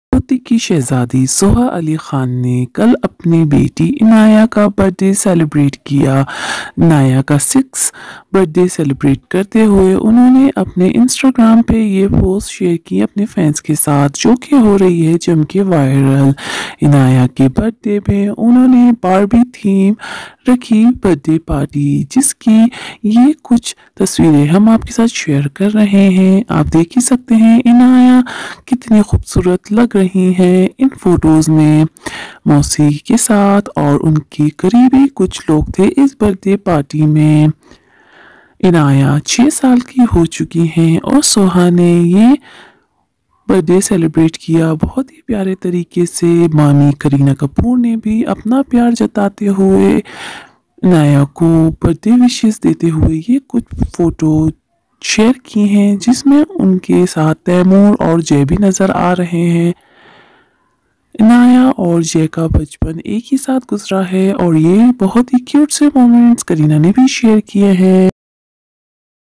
"Soha ali khan" Celebrate her Daughter Inaaya 🎂 Birthday 🥳
Actress sohalia khan Celebrate her daughter inaaya birthday
kareena kapoor share post for inaaya
sohalia khan patoti princess
birthday girl anaaya and taimur ali khan cute moments
celebrity's life